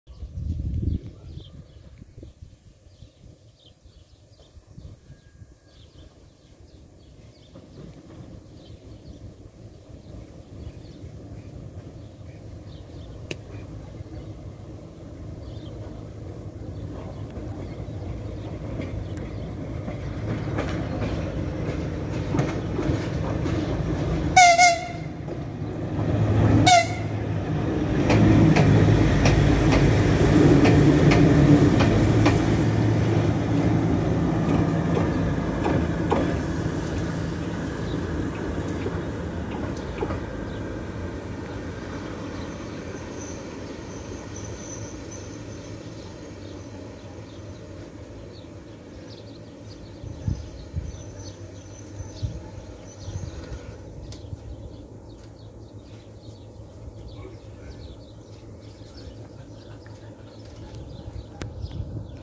Электровоз ВЛ-10У-631 после средного ремонта прибывает на ст. Масис, Армения - locomotive VL-10U-631
Электровоз ВЛ-10У-631 после средного ремонта прибывает на ст. Масис Армения

The VL-10U-631 electric locomotive, after a minor repair, arrives at the station. Masis Armenia